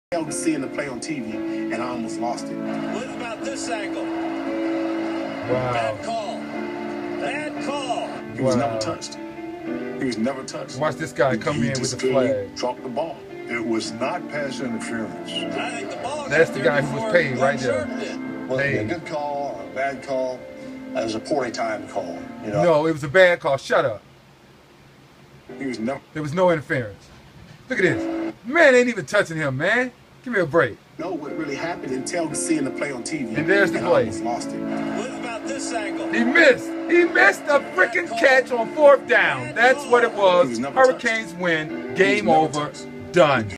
The Night The NCAA F#ckd The Miami Hurricanes!
For 8 seconds in the 2003 Fiesta Bowl, after double overtime,
The Hurricanes had won the game , Then a referee's &nbsp; whistle and a FAKE PASS INTERFERENCE CALL ...and the rest is HISTORY. WE KNOW WHO REALLY WON!. VEGAS! ...NAH THE U WON THAT Championship !